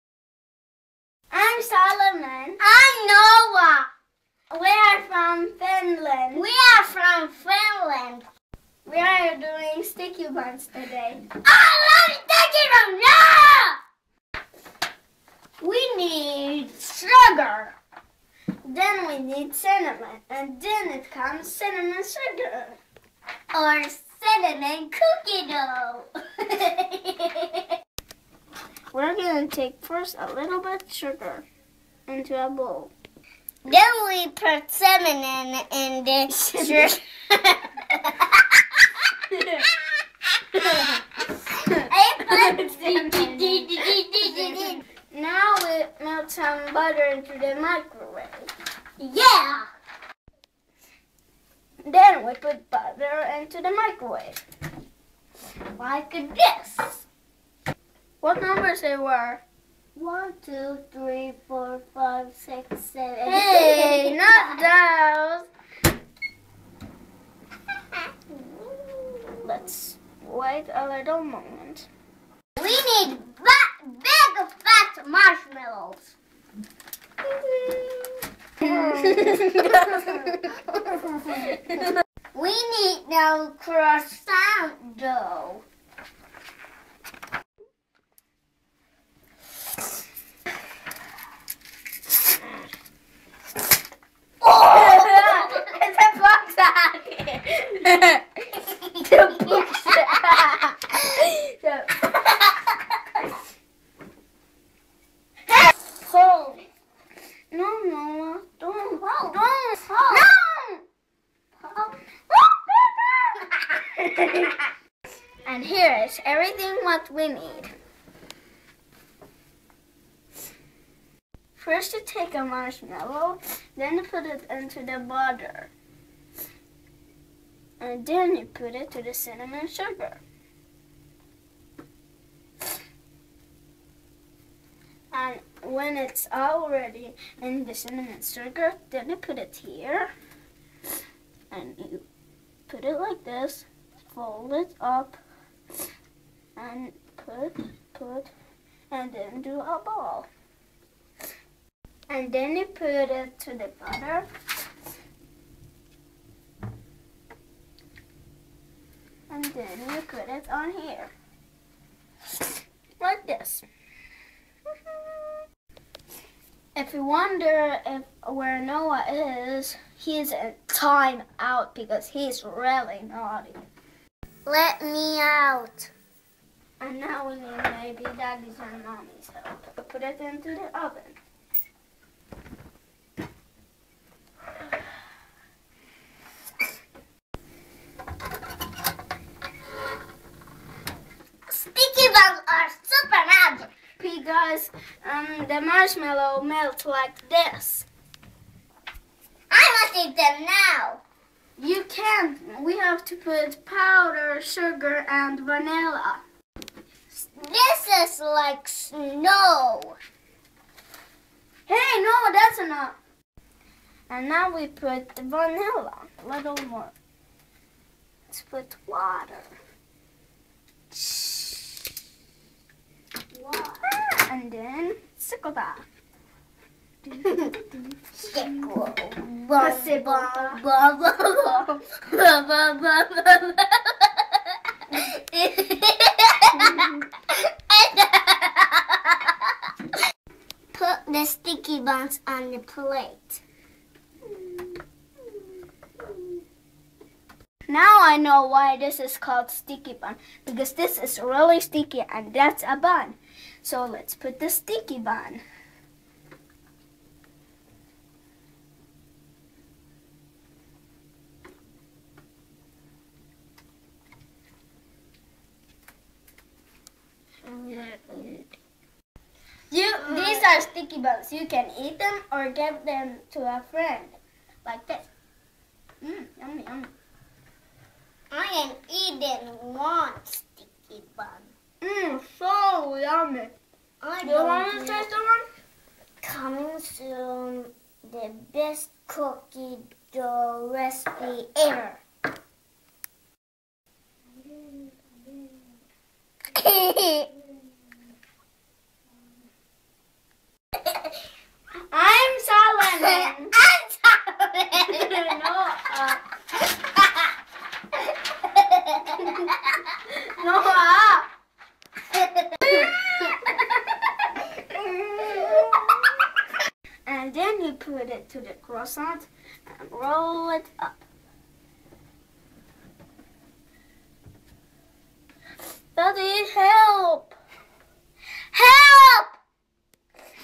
Salo and Noah: How to Make Sticky Buns
Enjoy Deliciously Easy Marshmallow Sticky Buns.  Brothers Salomon (8) & Noah (5) from Finland show how to make Sticky Buns. Easy enough for kids to bake, but decadent enough for their parents to serve to guests.  Video made with Nokia N95 8GB.

Ingredients:
Package of Refrigerated Croissant dough (6 per package)
Marshmallows (As many as buns)
Sugar (Approx 1/3 cup/ 1 dl)
Cinnamon (Approx 1/2 TB)
Butter/ Margarine (Approx1/2 cup/ 1.5 dl)
Powdered Sugar (Approx 1 cup (2.5 dl))
Vanilla Extract (1/2 TB)